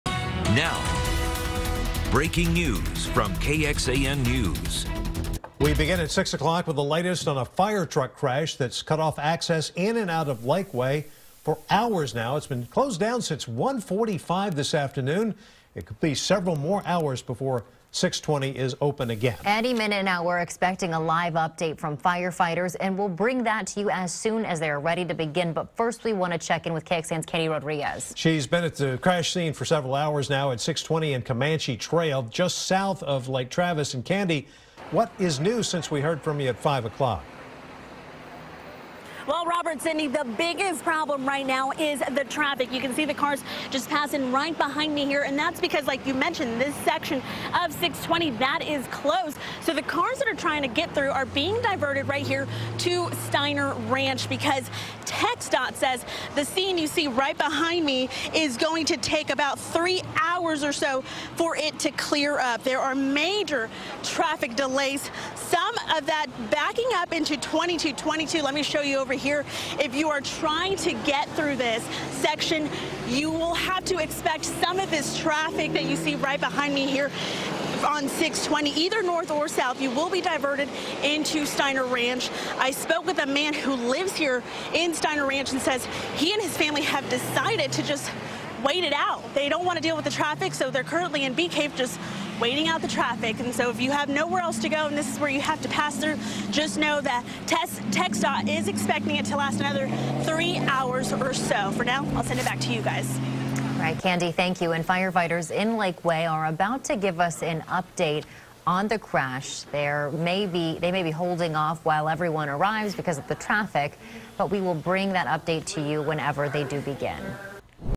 4 firefighters, 6 others hurt after fire truck crashes on FM 620
Four firefighters were injured after their fire truck was involved in a crash at Farm to Market 620 and Comanche Trail, just south of Lake Travis.